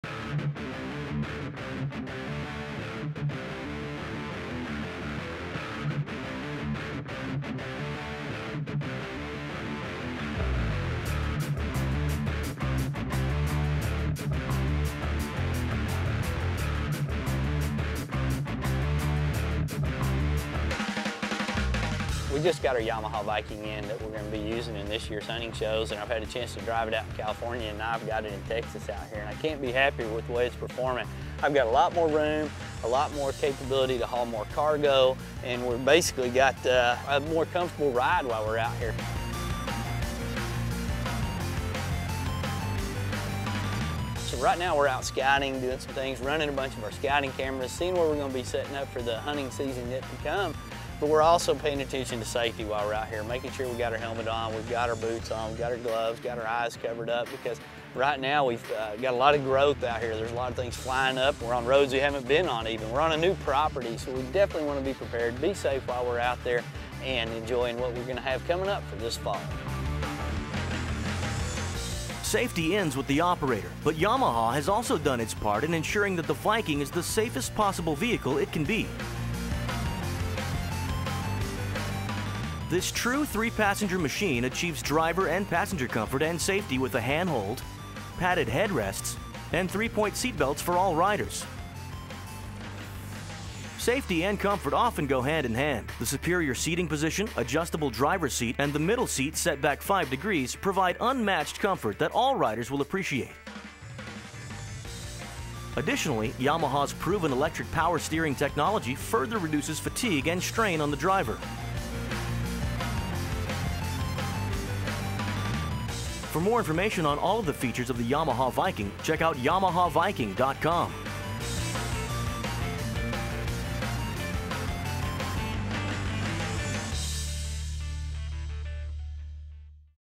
Putting the NEW Yamaha Viking to the Test at the Ranch
The true test for any side by side is to put it to work in the harsh environment like south Texas.  Hauling gear and getting to where you need to go is no problem with the Viking, and it does it in style and comfort.